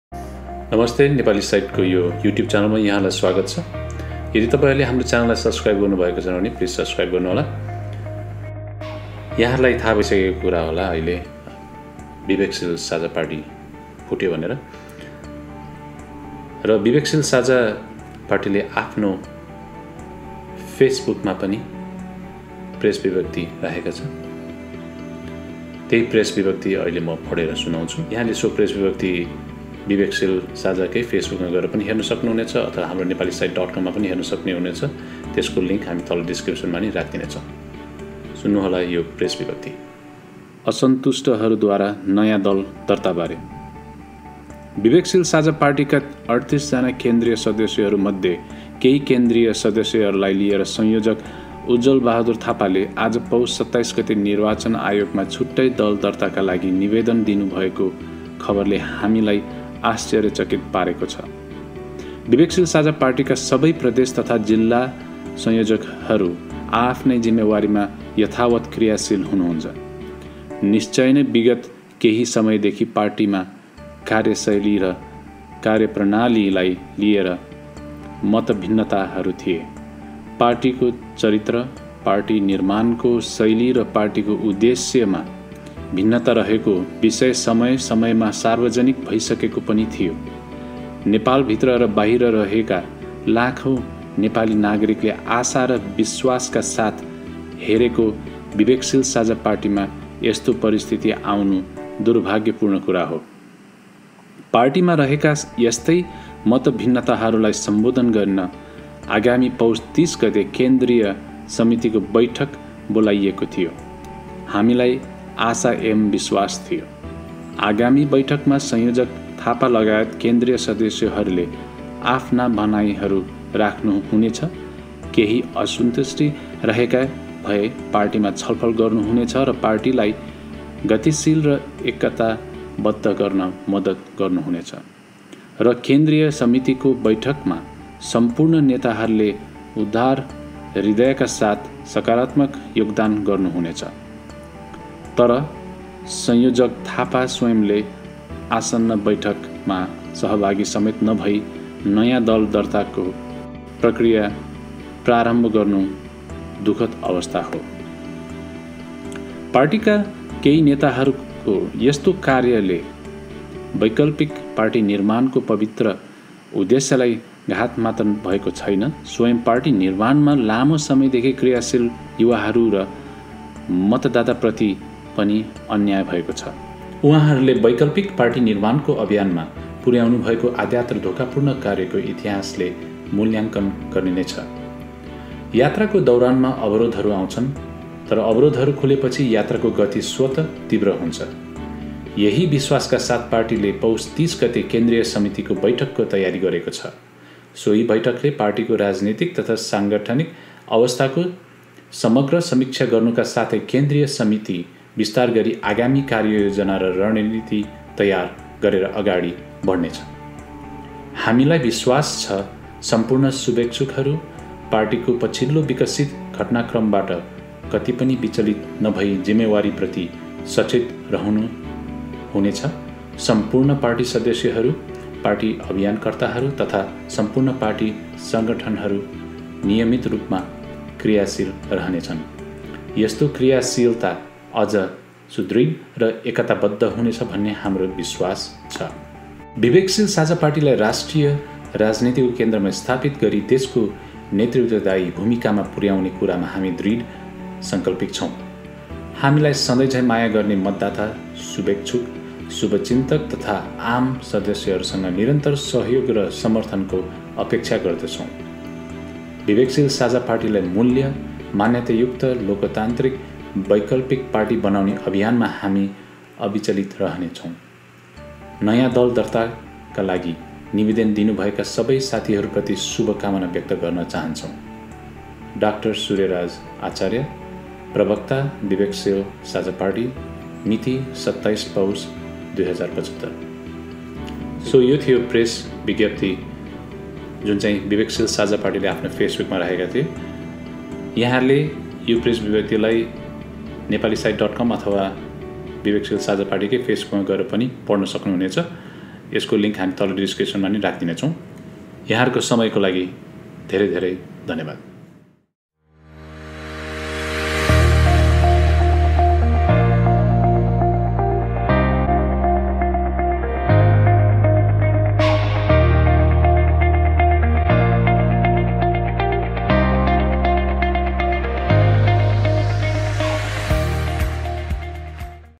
Bibeksheel Sajha Party Split | विवेकशील साझा पार्टी किन खत्तम !!!
In less than 2 years of unification, Bibeksheel Sajha Party has split on Friday that coincided with 297th Prithvi Jayanti, also known as National Unity Day.

According to the source, in an mutual and collaborative decision,  16 central committee members led by coordinator Ujwal Thapa filed an application with the Election Commission for a new party.